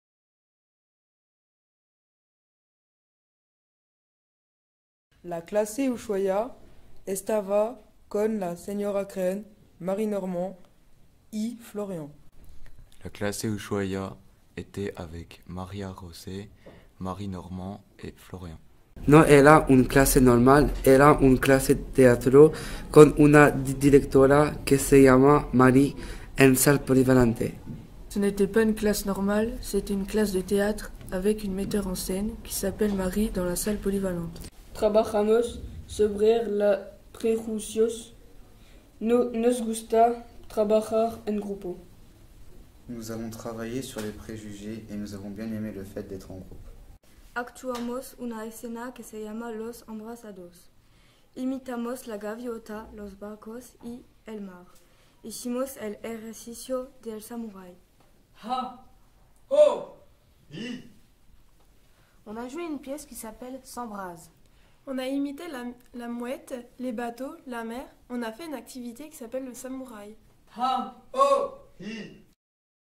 interview de l'activité théâtrale "S'embrasent"
Les élèves de la classe Ushuaïa expliquent en espagnole et en français se qu'ils ont fait en théâtre.